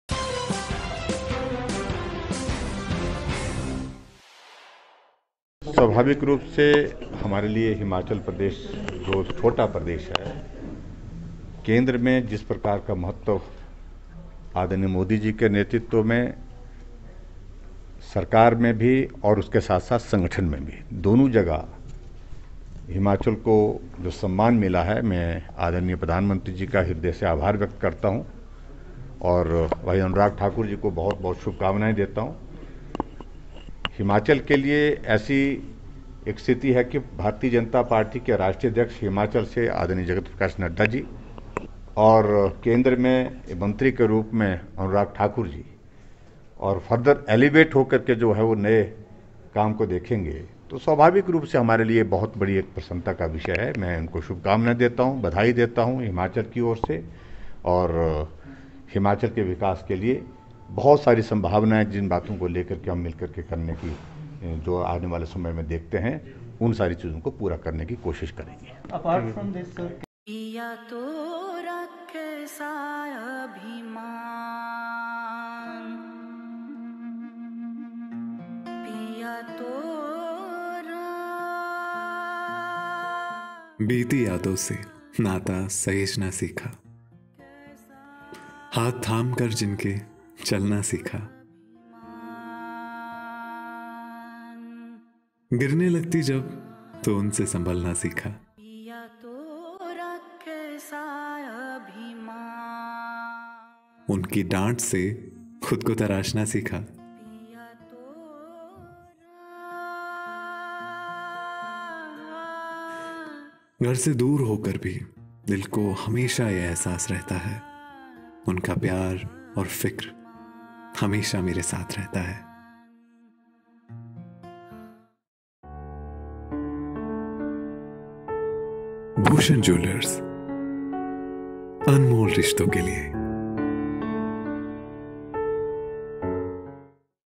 Anurag Thakur को CM Jairam Thakur ने दी बधाई एवं शुभकामनाएं | PM Modi Cabinet Expansion
The final list of 43 leaders joining PM Narendra Modi's cabinet has come out. Many big names have been given place in this list. The big news for Himachal Pradesh is that the name of Minister of State for Finance Anurag Thakur is also included in this list. Anurag has got a place in the cabinet. On the other hand, CM Jai Ram Thakur has also congratulated and congratulated Anurag Thakur for the promotion.